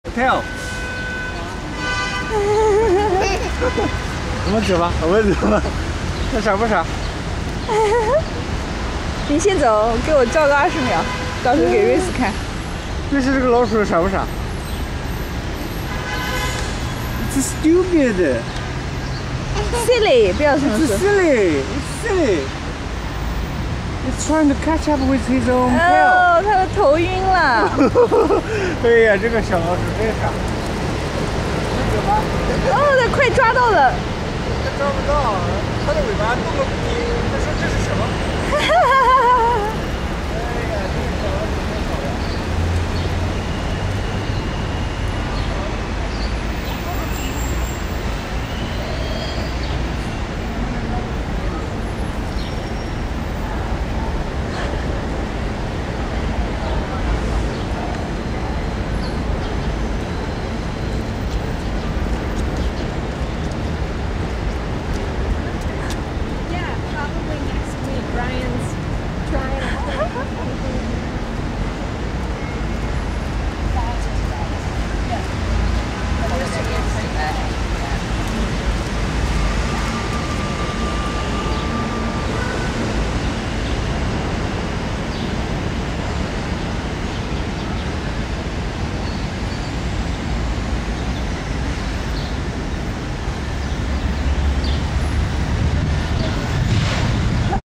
little mouse chasing its own tail
My 3-year-old daughter was really amused by this cute little mouse in New York Central Park.  We had no idea whether it's playing with its own tail or it's really thinking of its own tail as an earthworm.  We had no idea how long the mouse had been chasing its own tail and when it would stop, either by itself or by someone.